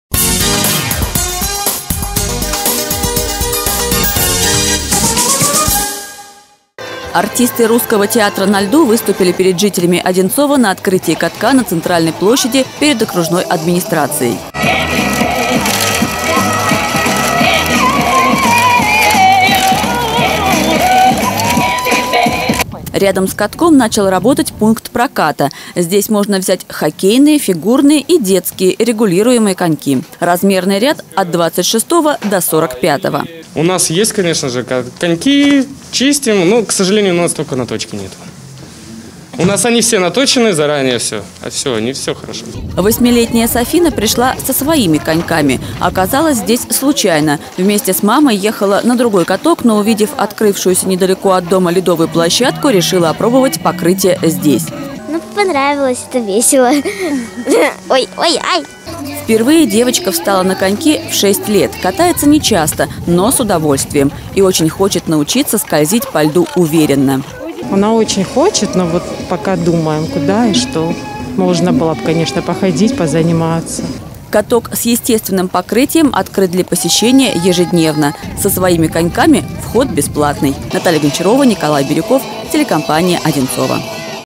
«Баранка» ждёт!
Каток с естественным покрытием открыли на центральной площади Одинцова. После красочного шоу ледового театр покататься на любимой «баранке» смогли все желающие.